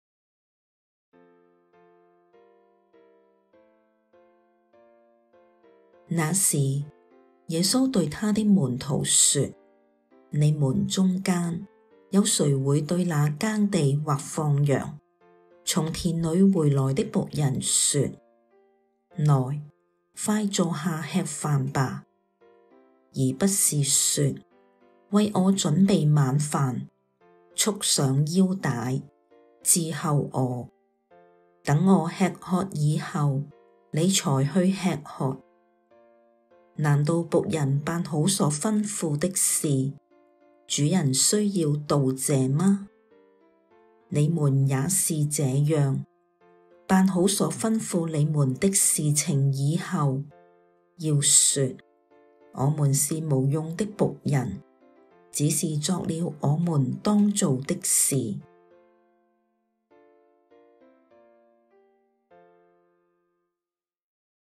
路加福音 17:7-10
路加福音 17:7-10（門徒應有的態度）

那時，耶穌對祂的門徒說：「你們中間，有誰會對那耕地或放羊，從田裡回來的僕人說：『來，快坐下吃飯吧』？而不是說：『為我準備晚飯，束上腰帶，伺候我；等我吃喝以後，你才去吃喝』？難道僕人辦好所吩咐的事，主人需要道謝嗎？你們也是這樣，辦好所吩咐你們的事情以後，要說：『我們是無用的僕人，只是作了我們當做的事。』」